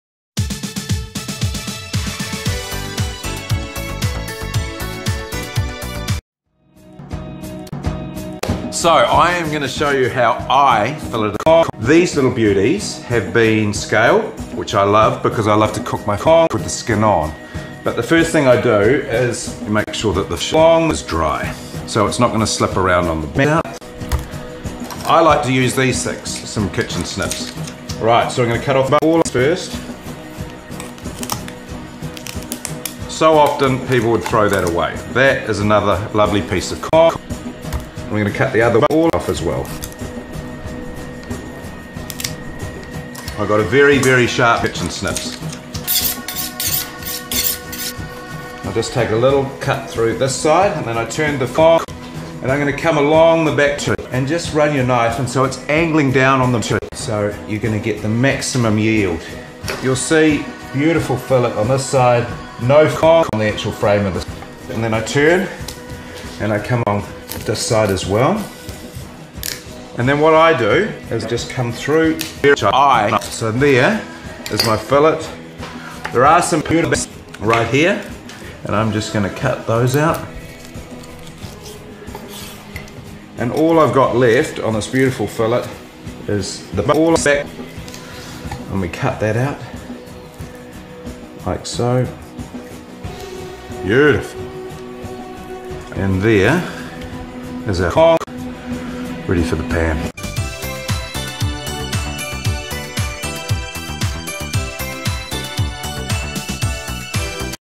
FIllet a fish ytp
New Zealand chef Al Brown teaches us how to fillet not just an ordinary fish. He shows us how to do it in his own way with his exclusive techniques.